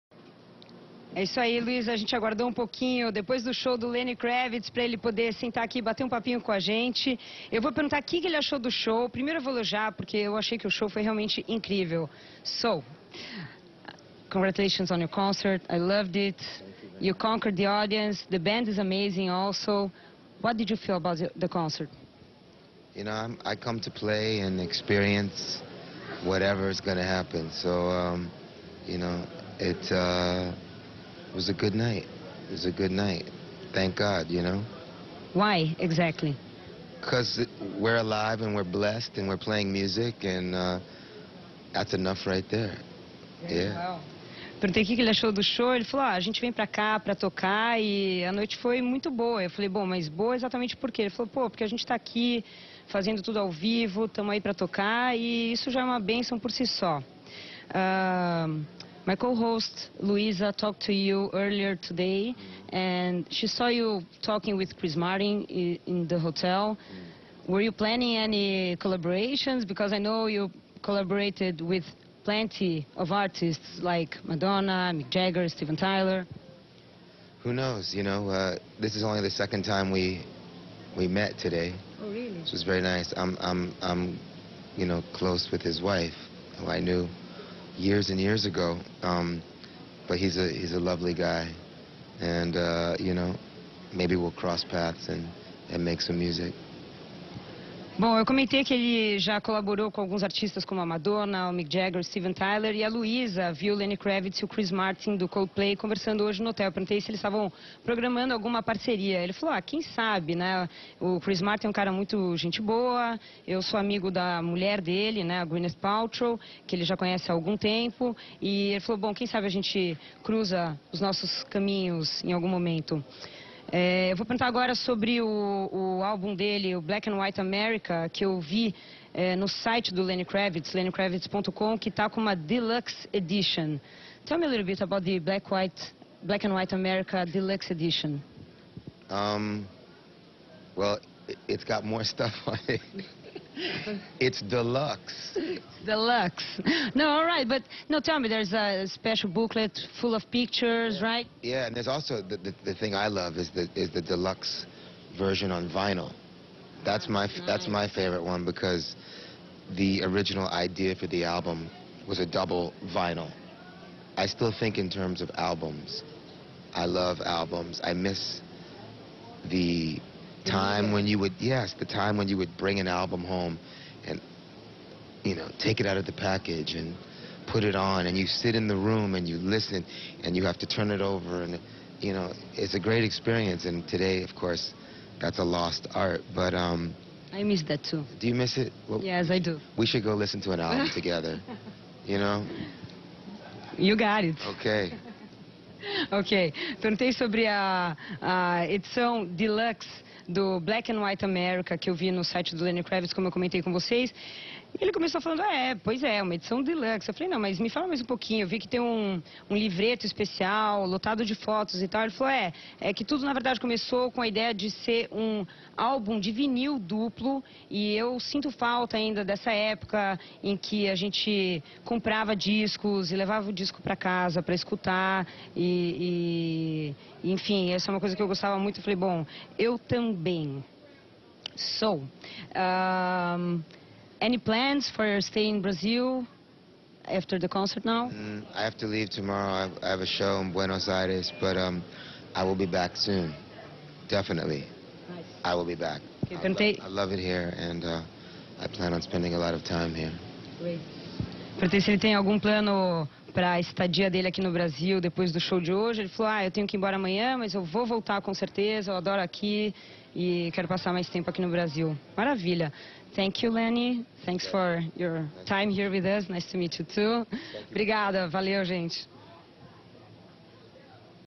Lenny Kravitz sobre o Brasil: "Eu voltarei" [LEGENDADO]
Didi Wagner conversa com Lenny Kravitz em seu camarim. Ele fala que foi uma "boa noite", comenta que sente falta dos tempos de discos de vinil e diz que vai voltar ao Brasil.